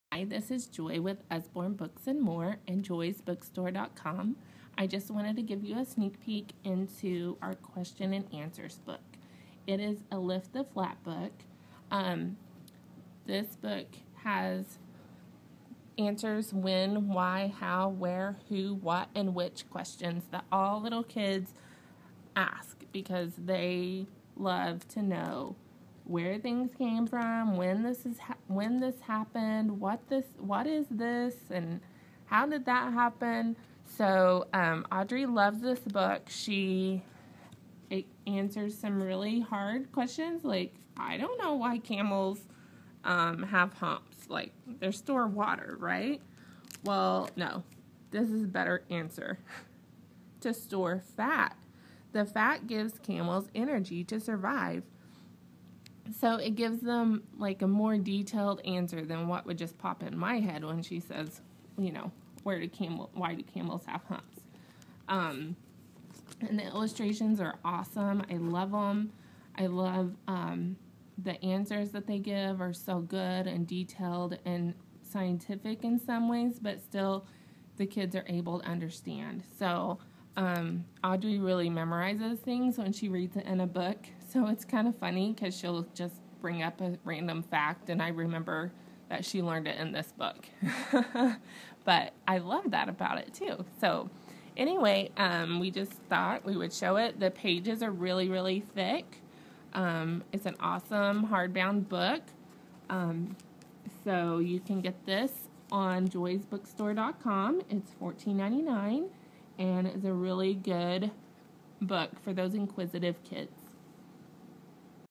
Lift Flap Question and Answers Book - joysbookstore.com
Awesome book for the inquisitive child! My three year old loves this book!